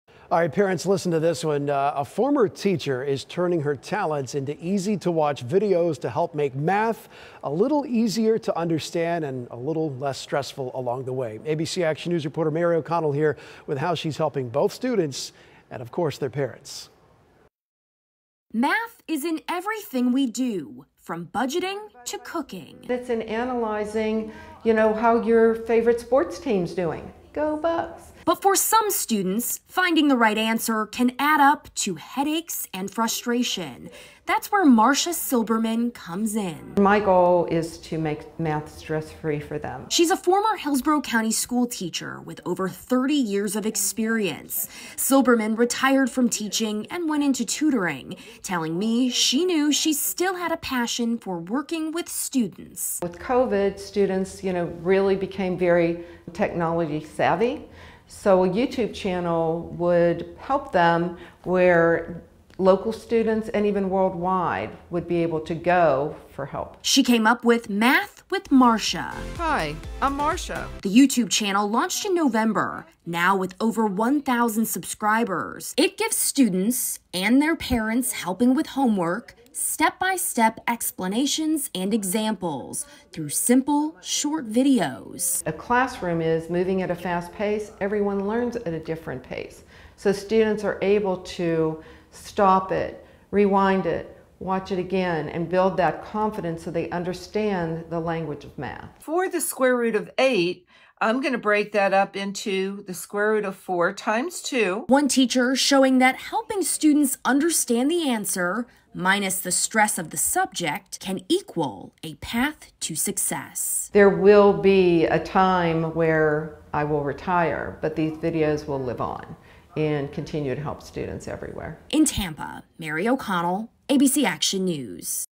Former teacher launches YouTube channel to help students with math
Math might not be every student’s favorite subject, but a former Hillsborough County teacher turned her knowledge into easy-to-watch videos to make math simple and stress-free.

Marcia Silberman took her talents to the virtual world, and she hopes it will help kids find the right answer with less frustration.

“Math really is everywhere. It’s in personal finance. It’s in budgeting. It’s in fractions and cooking,” said Silberman. “It’s in analyzing how your favorite sports team’s doing.”